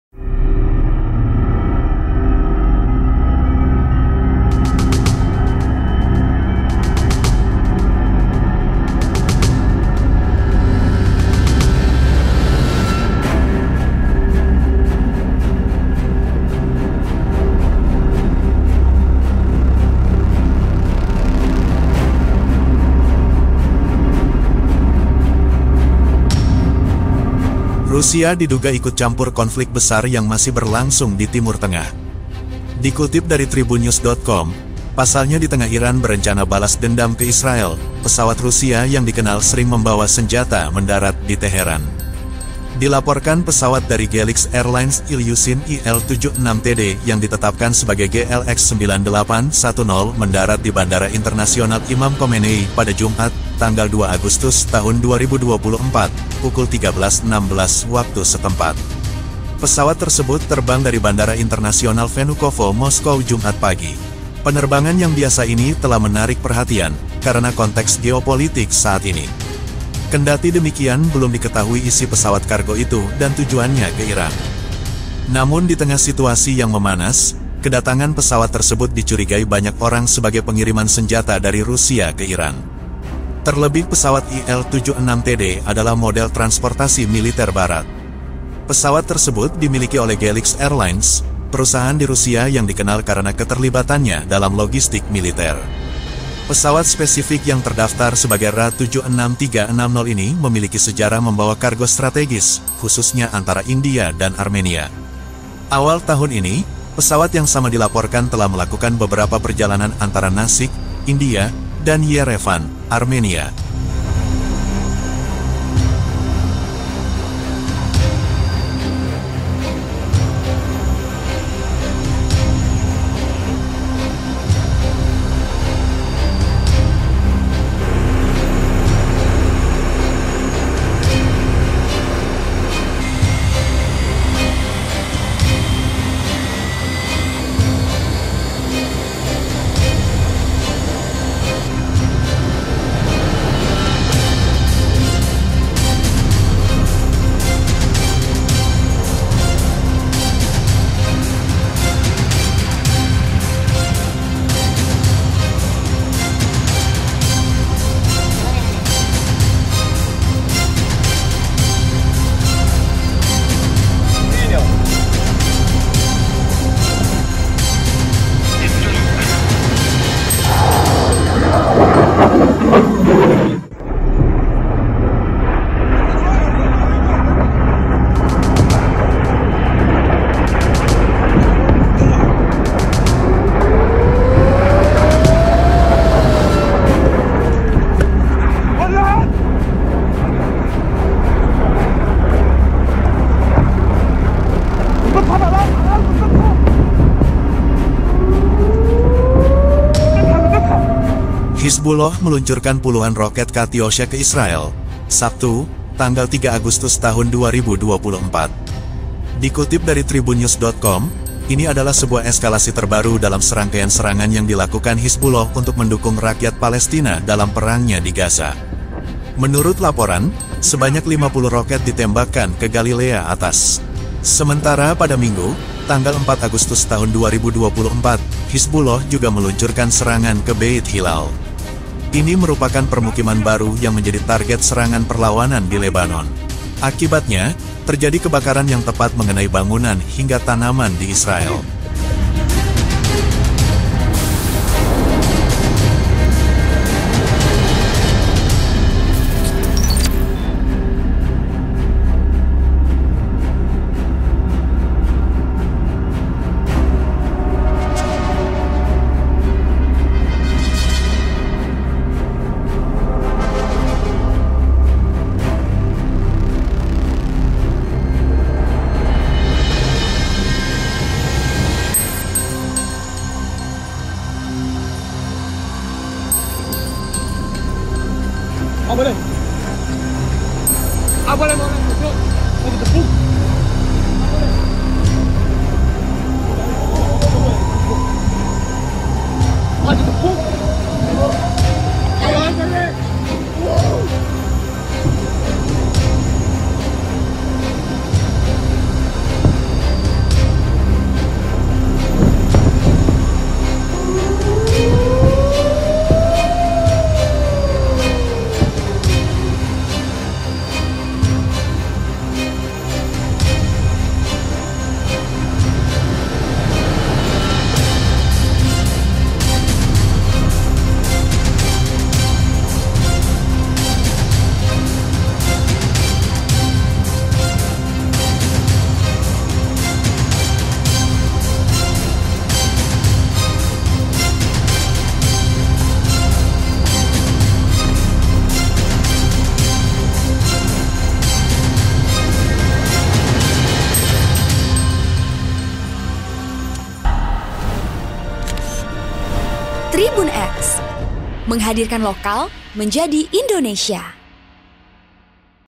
®️🔴UPDATE Hari ke-303❗Pesawat Militer Rusia Tiba di Iran Serta Rudal-Rudal Hizbullah Hantam Israel
Rusia diduga ikut campur konflik besar yang masih berlangsung di Timur Tengah.

Dikutip dari Tribunnews.com, pasalnya di tengah Iran berencana balas dendam ke Israel, pesawat Rusia yang dikenal sering membawa senjata mendarat di Teheran.

Dilaporkan pesawat dari Gelix Airlines Ilyushin IL-76TD yang ditetapkan sebagai GLX9810 mendarat di Bandara Internasional Imam Khomenei pada Jumat (2/8/2024) pukul 13.16 waktu setempat.

Pesawat tersebut terbang dari Bandara Internasional Vnukovo Moskow Jumat pagi.

Penerbangan yang biasa ini telah menarik perhatian, karena konteks geopolitik saat ini.

Hizbullah meluncurkan puluhan roket Katyusha ke Israel, Sabtu (3/8/2024).

Dikutip dari Tribunnews.com, ini adalah sebuah eskalasi terbaru dalam serangkaian serangan yang dilakukan Hizbullah untuk mendukung rakyat Palestina dalam perangnya di Gaza.

Menurut laporan, sebanyak 50 roket ditembakkan ke Galilea Atas.

Sementara pada Minggu (4/8/2024), Hizbullah juga meluncurkan serangan ke Beit Hilal.

Ini merupakan permukiman baru yang menjadi target serangan perlawanan di Lebanon.

Program: -
Host: -
Editor Video: Tito Ramadhani
Uploader: -